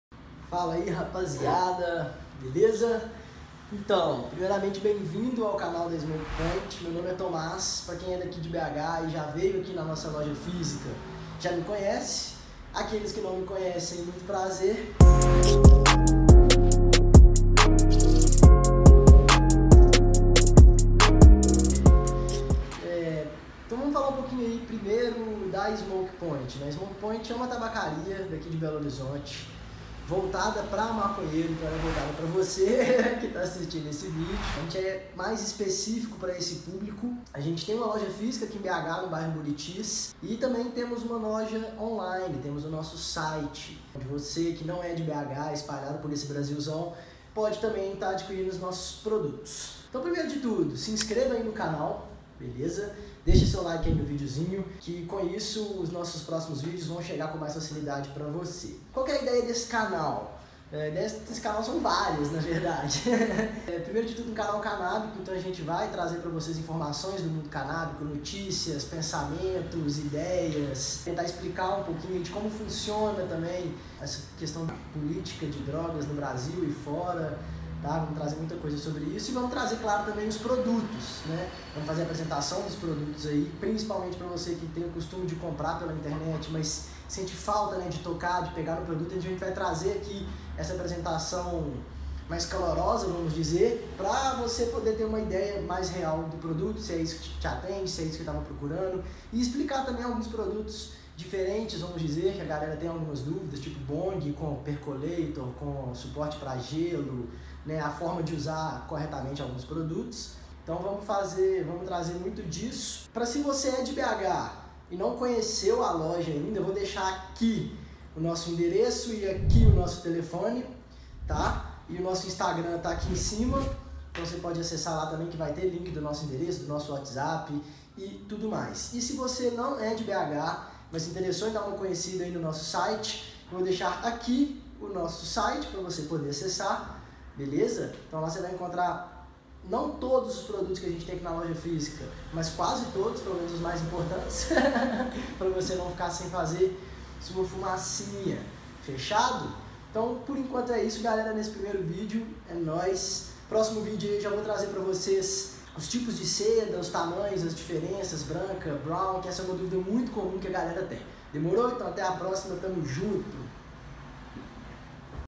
BEM VINDOS AO CANAL DA SMOKE POINT
Fala galera! Eu sou o Thomaz da Smoke Point e sejam muito bem vindos ao canal. O cena canábica precisa de mais voz, representatividade e informação de fácil acesso. É por isso que to aqui e vou trazer pra vocês conteúdo exclusivamente canábico: notícias, informações, produtos e dicas malandras.

Se liga em nosso blog!

Fiquem ligados por aqui, o mundo canábico precisa de mais voz e mais representatividade, vem com a gente nessa pra fortalecer a cena.